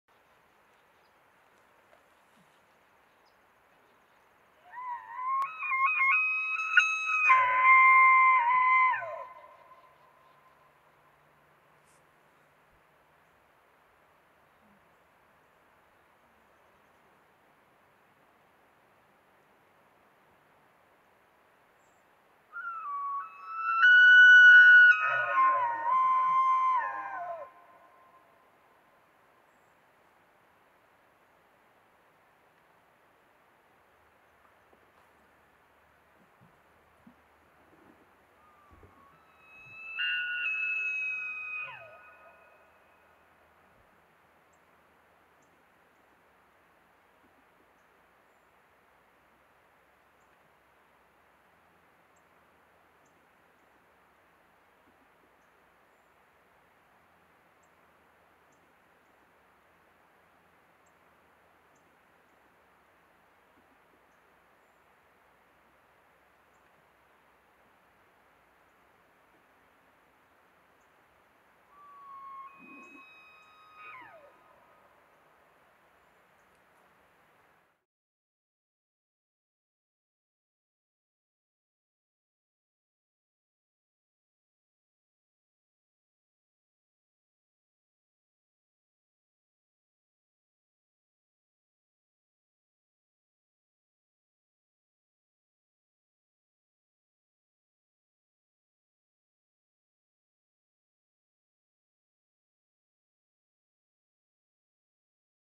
Elk Bugling in the Great Smoky Mountains National Park
There is nothing like the sound of Elk bugling in the Great Smoky Mountains. Video filmed in Cataloochee Valley - Great Smoky Mountains National Park.